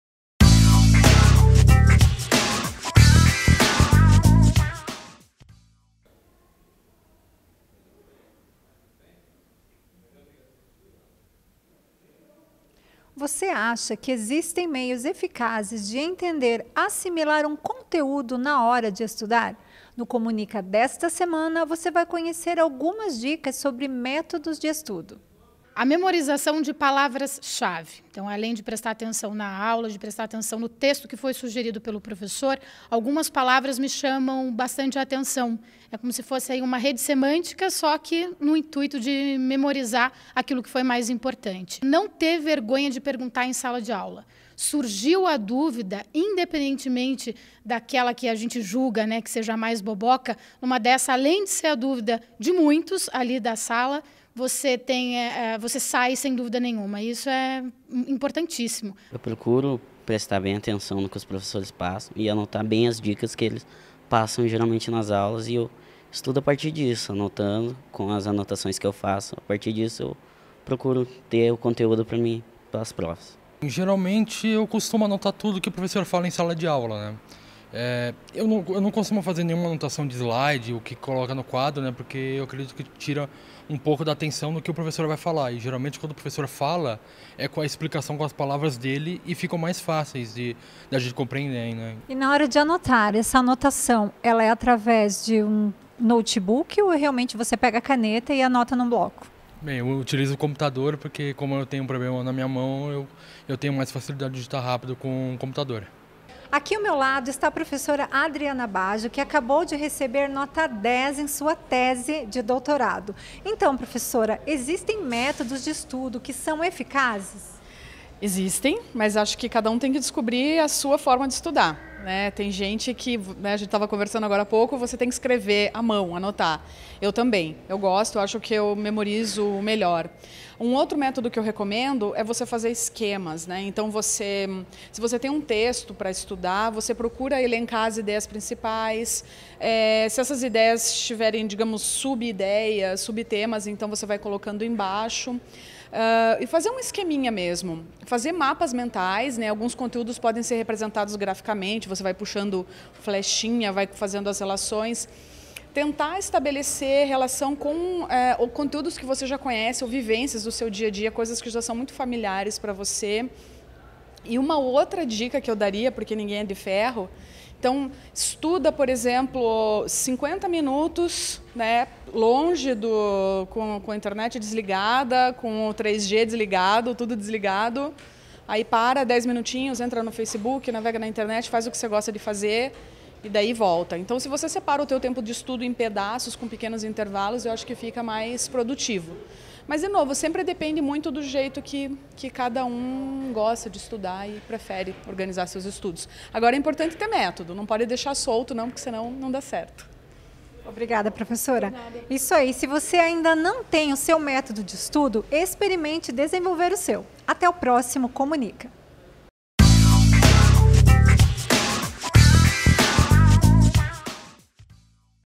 Comunica - Métodos de Estudo
No Comunica desta semana você vai conhecer alguns métodos de estudo que são eficazes na hora de fixar o conteúdo.